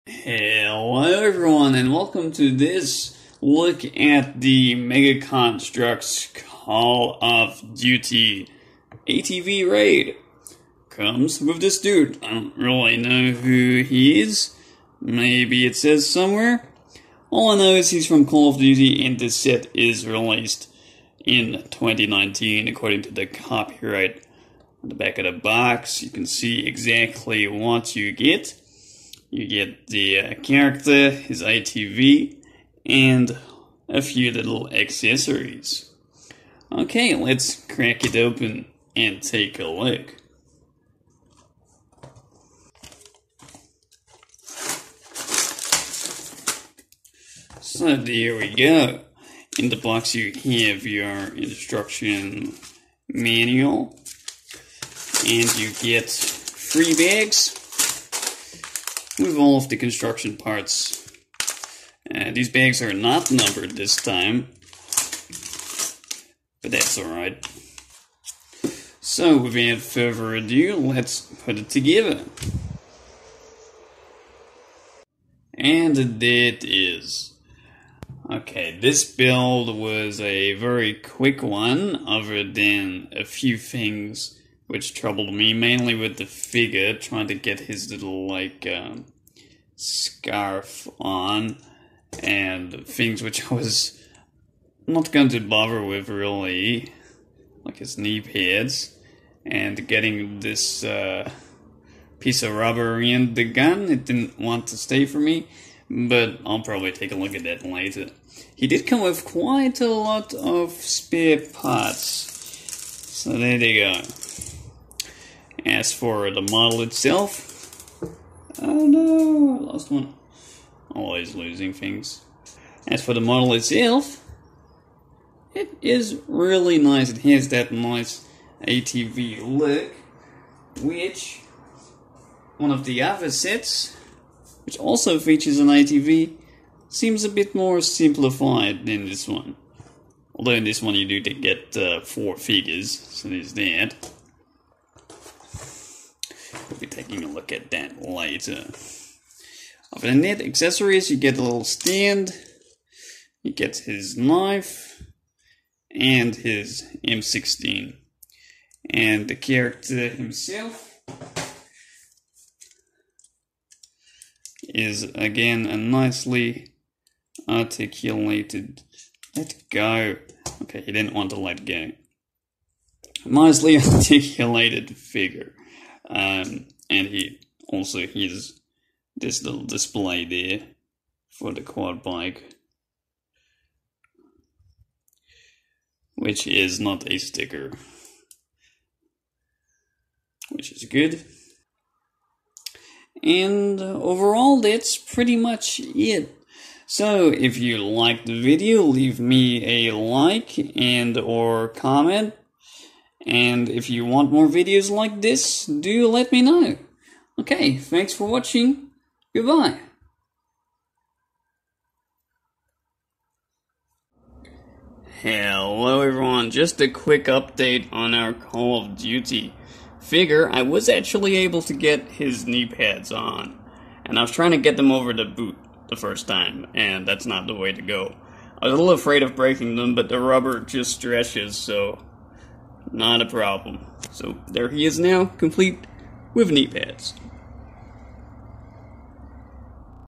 Primeval Reviews #3 Mega Construx CoD Black Ops 4 ATV [GCN95]
Taking a look at this Mega Construx Call of Duty set